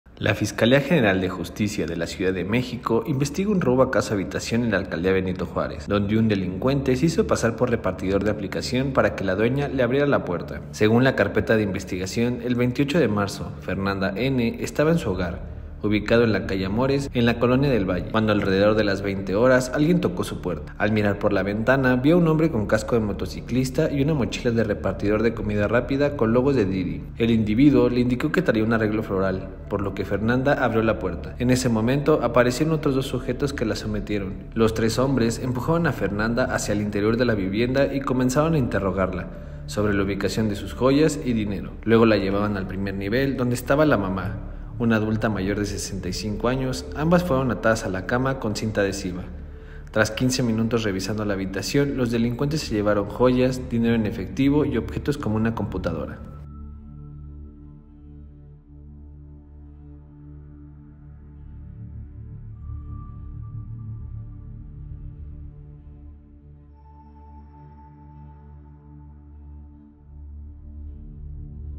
Se hacen pasar por repartidores de app para robar una casa en BJ
La Fiscalía General de Justicia (FGJ) de la Ciudad de México investiga un robo a casa habitación en la alcaldía Benito Juárez, donde un delincuente se hizo pasar por repartidor de aplicación para que la dueña le abriera la puerta.